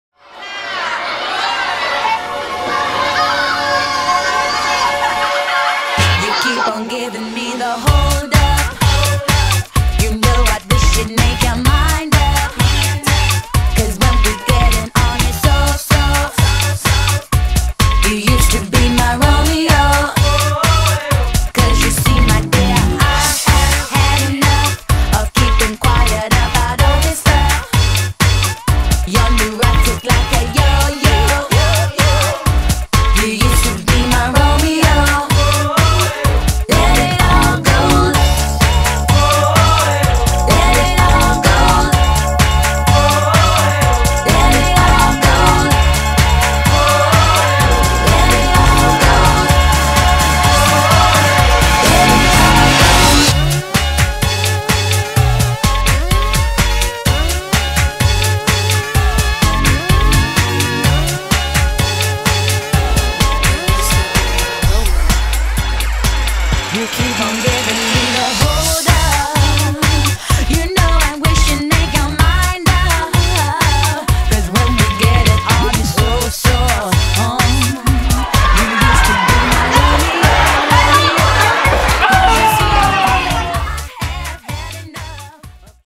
What can an eight year old create in 2 hours?
A whole lot of fun that's what!  Check out adults playing games created for the Global Cardboard Challenge.